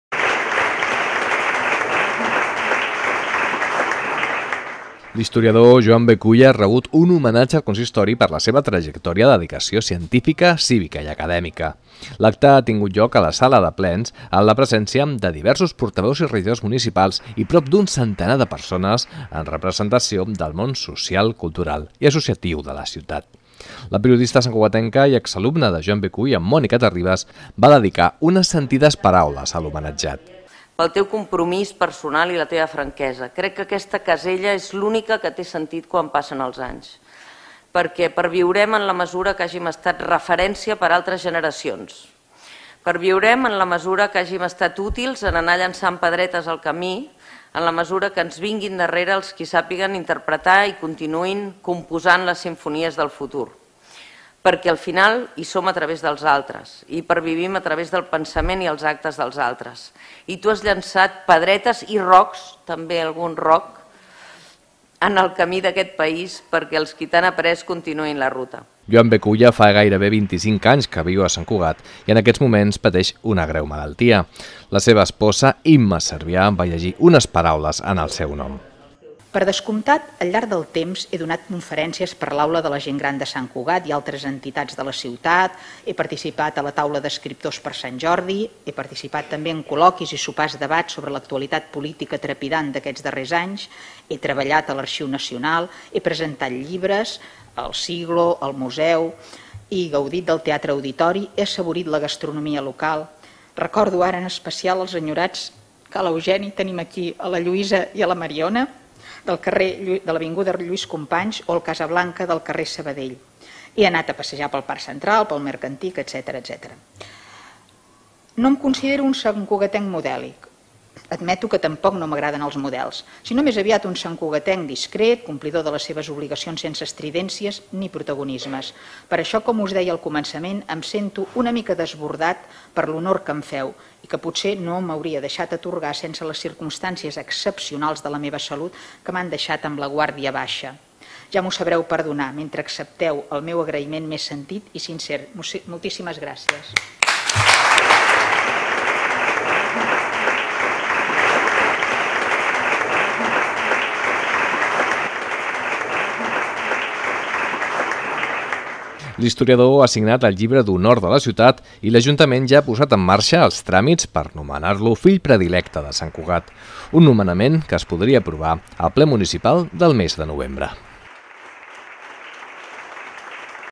Sant Cugat homenatja l’historiador Joan B. Culla i inicia els tràmits per nomenar-lo fill predilecte
L’historiador Joan B. Culla ha rebut un homenatge al consistori per la seva trajectòria de dedicació científica, cívica i acadèmica. L’acte ha tingut lloc a la sala de plens amb la presència de diversos portaveus i regidors municipals i un centenar de persones en representació del món social, cultural i associatiu de la ciutat. La periodista santcugatenca i exalumna de Joan B. Culla, Mònica Terribas, va dedicar unes sentides paraules a l'homenatjat.